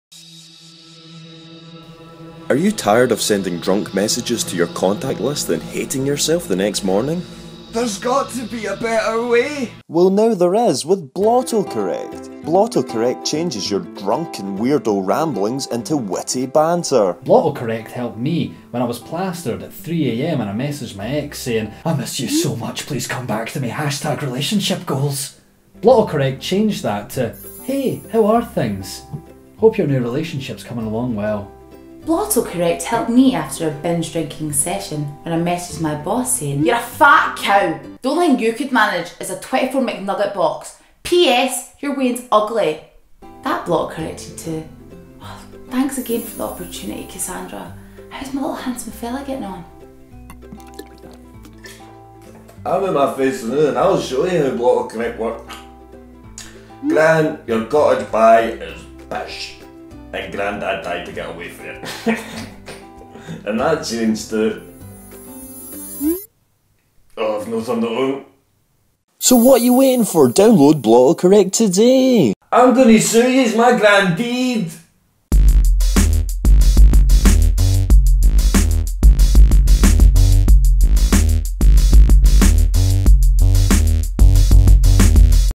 Blotto Correct - The App You Need!
If you've had a few drinks and you're thinking of texting your boss to tell her that she's a cow, then Blotto Correct is the app for you!

If you drunk texted your gran to let her know her cottage pie is rubbish, Blotto Correct, the drunk autocorrect app, will change it to something nice like: "Hey Gran, love you, see you at the Bingo tomorrow".

No longer will you check your phone in the morning and realise you have 100% ruined your life! Thanks, Blotto Correct.